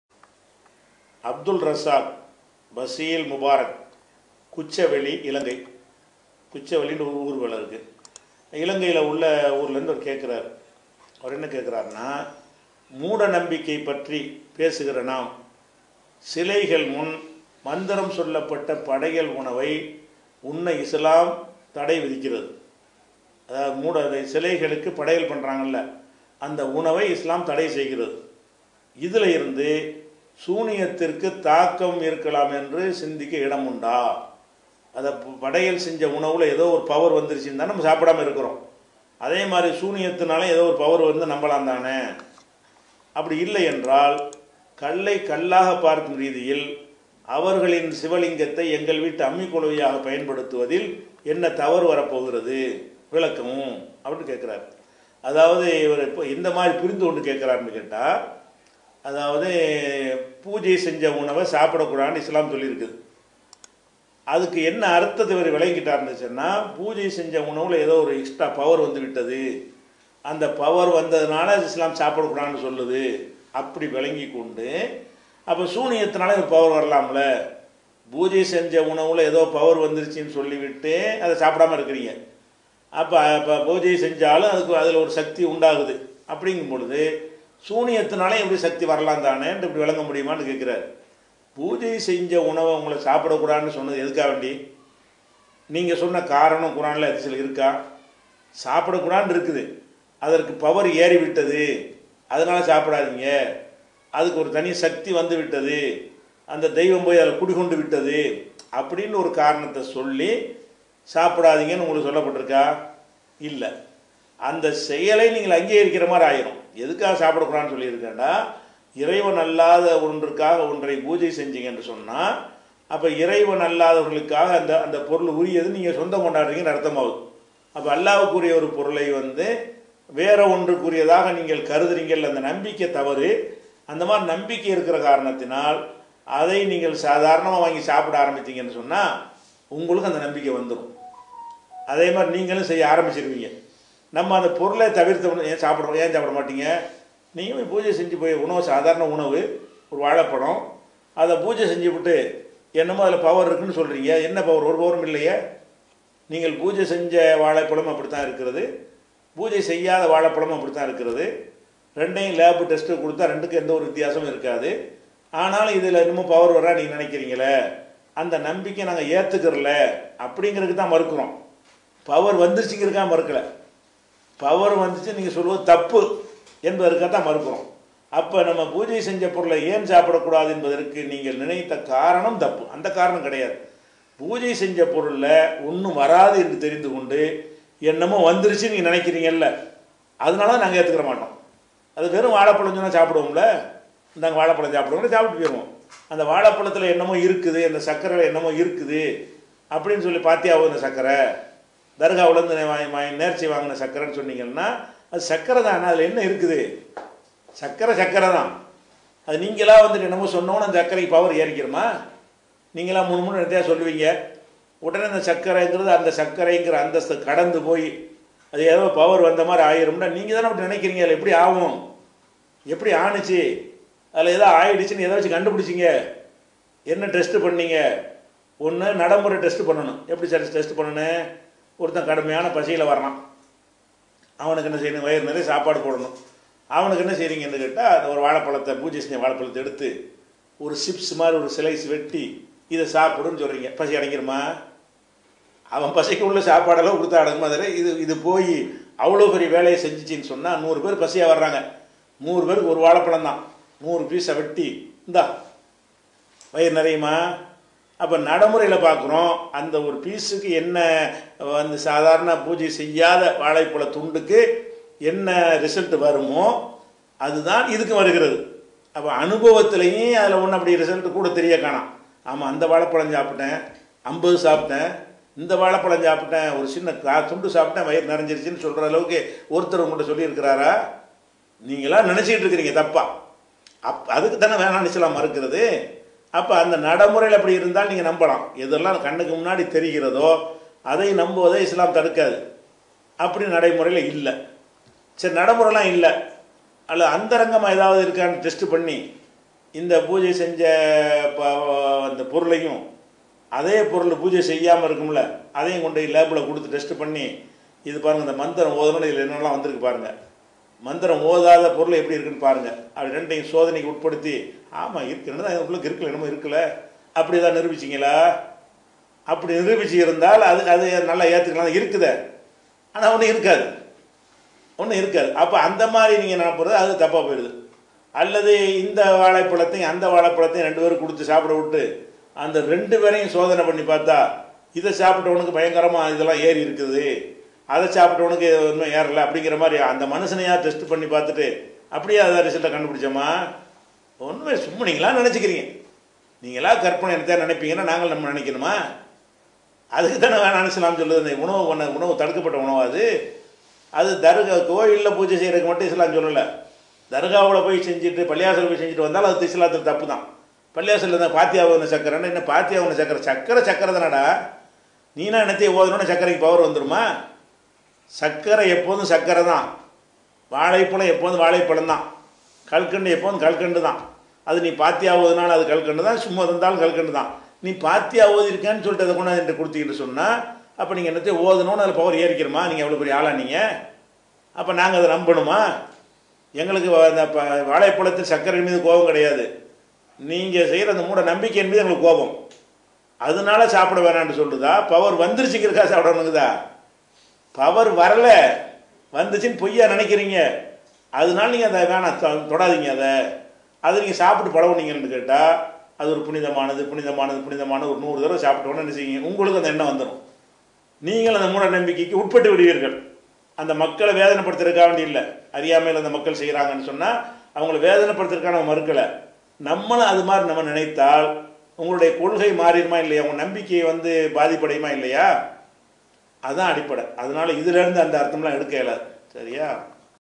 சூனியத்திற்கு சக்தி உண்டு என்று நம்பலாமா?
சிலைகளுக்கு படையல் செய்ததை சாப்பிடக்கூடாதென்றால் சூனியத்திற்கு சக்தி உண்டு என்று நம்பலாமா?
வாட்ஸ் அப் கேள்வி பதில் 06/12/2020